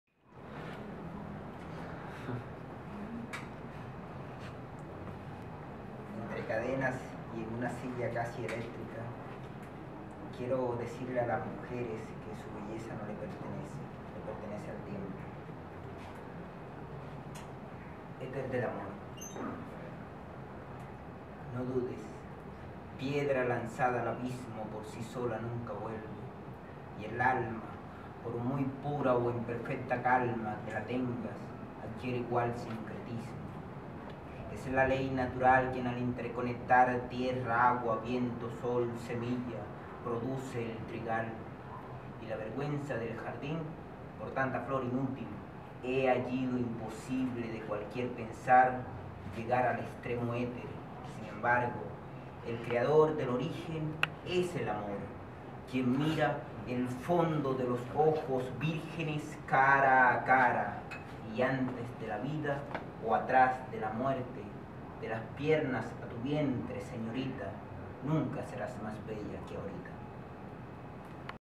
Éter del Amor (Poema) Jesús Espicasa
Poeta invitado al proyecto live session poetry de libido bar